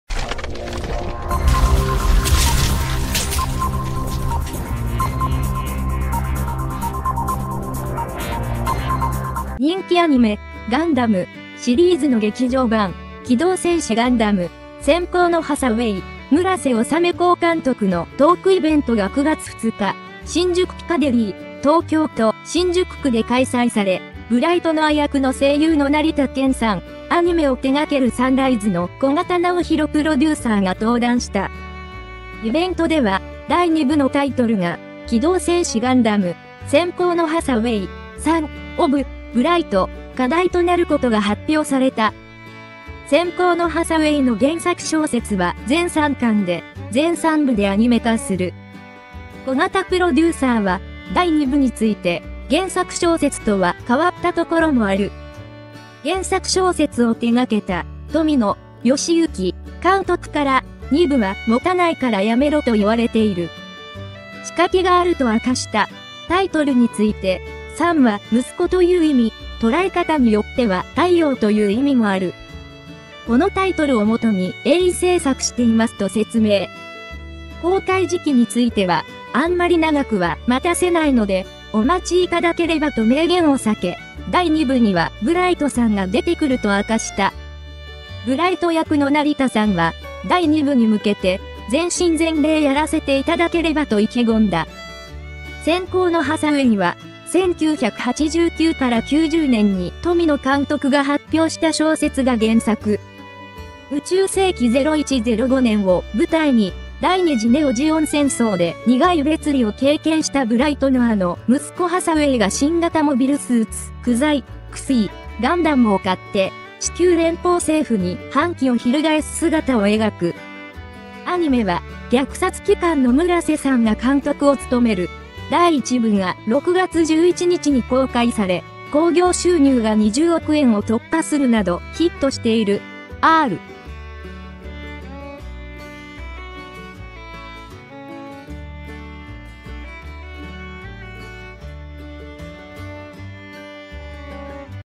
機動戦士ガンダム 閃光のハサウェイ：第2部「サン オブ ブライ」（仮題） タイトルの意味 仕掛け 原作小説と変えるところも
人気アニメ「ガンダム」シリーズの劇場版「機動戦士ガンダム 閃光のハサウェイ」（村瀬修功監督）のトークイベントが9月2日、新宿ピカデリー（東京都新宿区）で開催され、ブライト・ノア役の声優の成田剣さん、アニメを手がけるサンライズの小形尚弘プロデューサーが登壇した。イベントでは、第2部のタイトルが「機動戦士ガンダム 閃光のハサウェイ『サン オブ ブライト』（仮題）」となることが発表された。　「閃光のハサウェイ」の原作小説は全3巻で、全3部でアニメ化する。